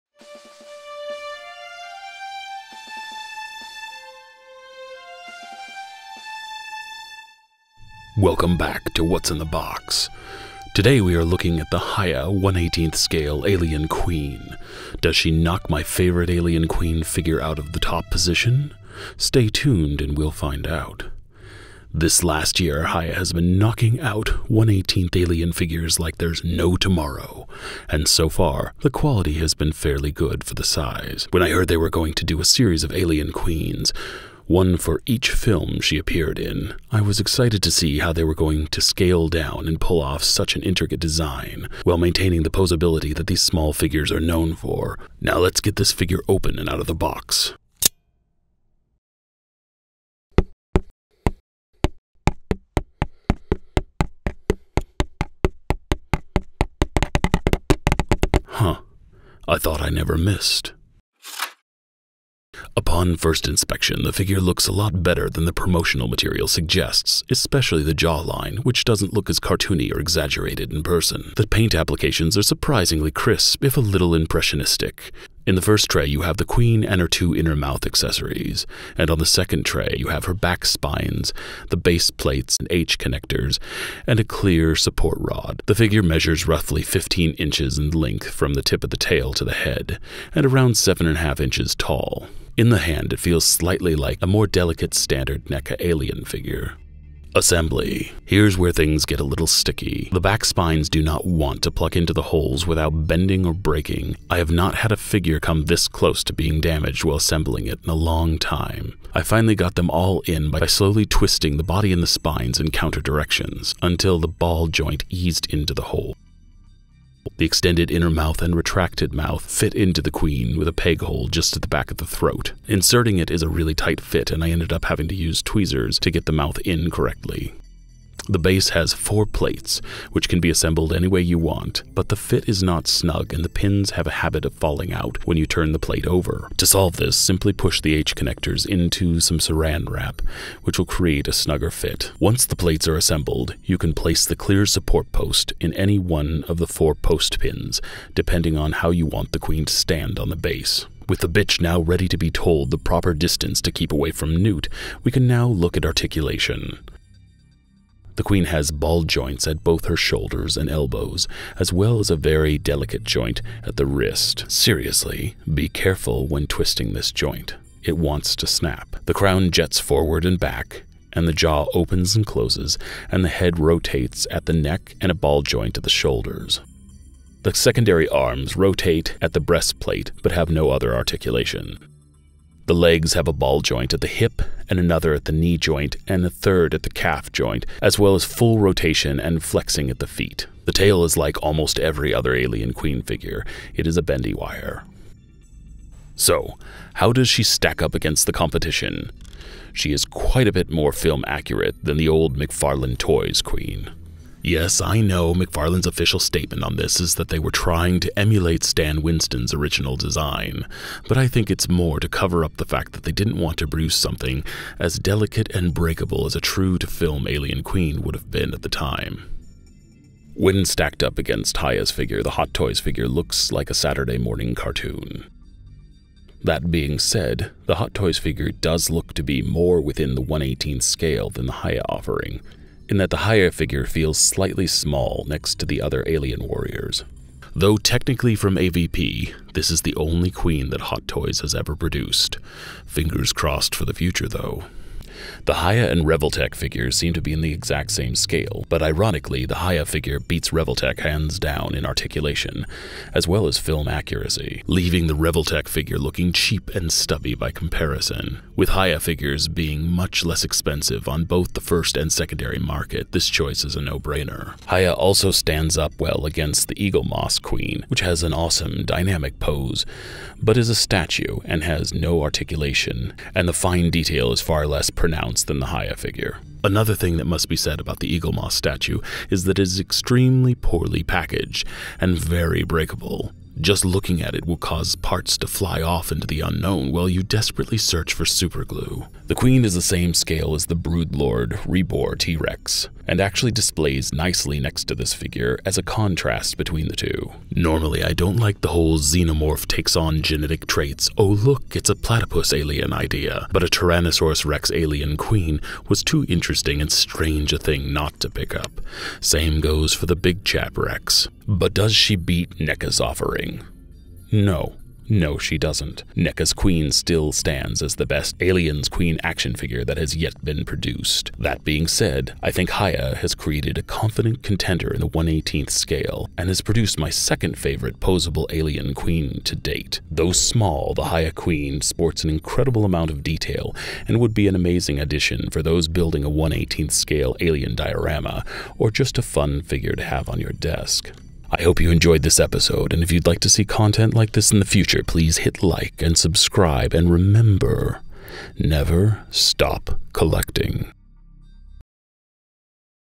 HIYA TOYS ALIENS QUEEN UN-BOXING AND REVIEW
Today we are un-boxing and reviewing the HIYA 1/18 scale Aliens Queen, does she knock my favorite alien queen figure out of the top position? This last year hiya has been knocking out 1/18th alien figures like there’s no tomorrow, and so far, the quality has been fairly good for the size and price. So let’s take a look at how this killer Queen stacks up against other previous Queen offerings over the years.